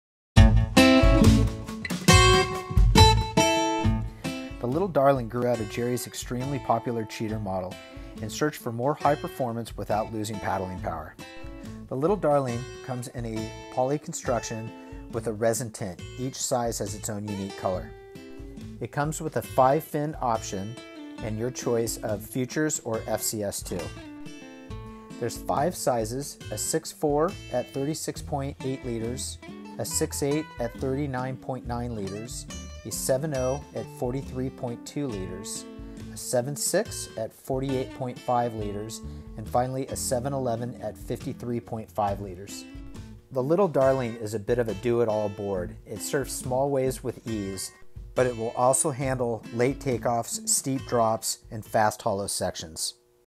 Gerry Lopez Little Darlin in Fusion Poly Construction
The LITTLE DARLIN grew out of Gerry's extremely popular Cheater in the search for more high performance without losing the paddle power. The Little Darlin' is a sweet, quick, easy-riding board that can take it to the juice with confidence, knowing it will catch most any wave and still handle late takeoffs, steep drops, and fast, hollow sections. A single to double concave gives the Little Darlin' speed and holding power when the juice comes down.